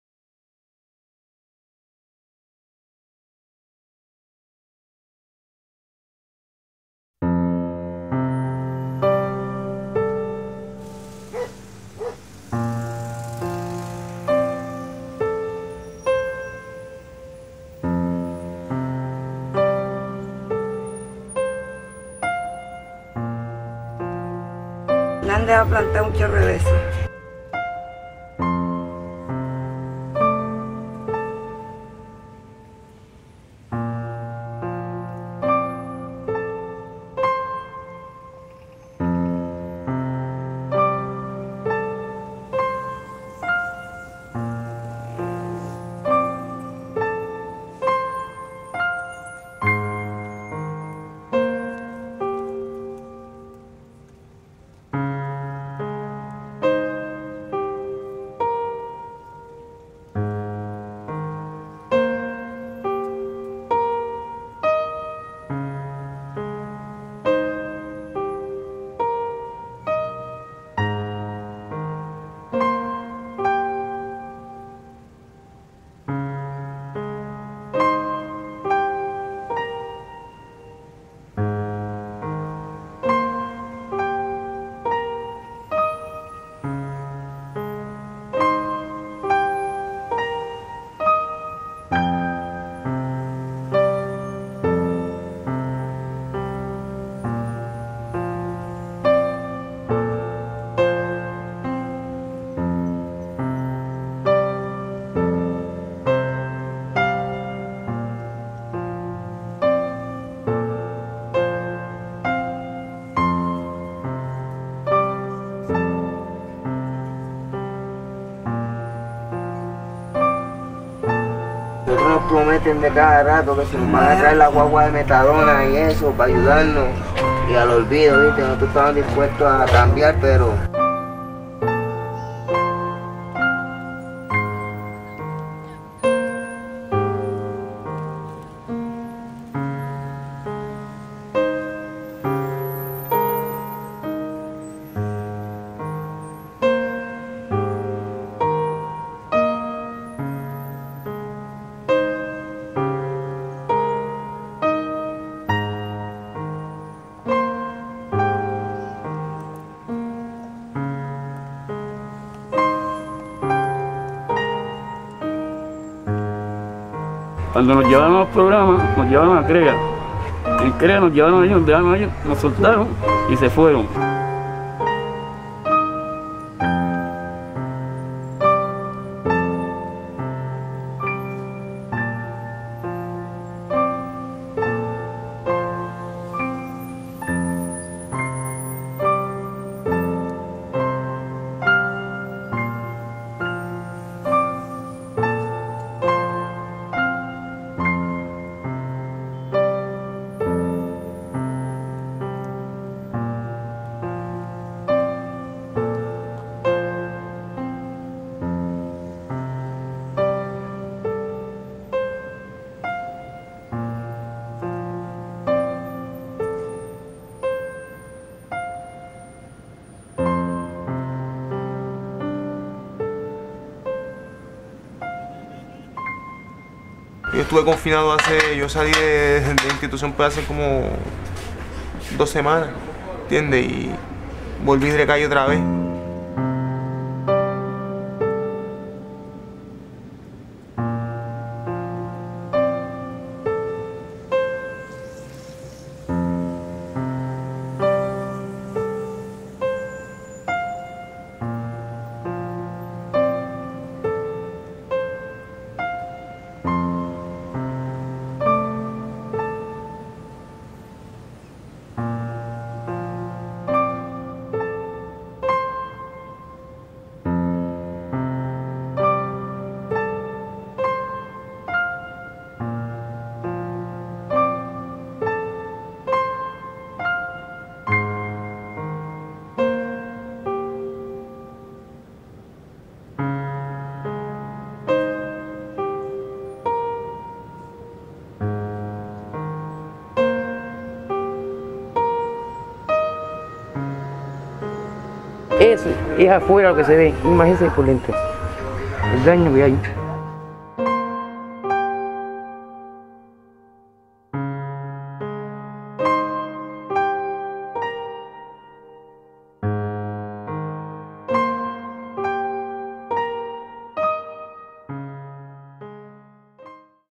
La cura y nosotros2
Una muestra de fotos que veran a continuación en la exposición del Fotografo y Profesor de Fotografía, Farrique Pesquera.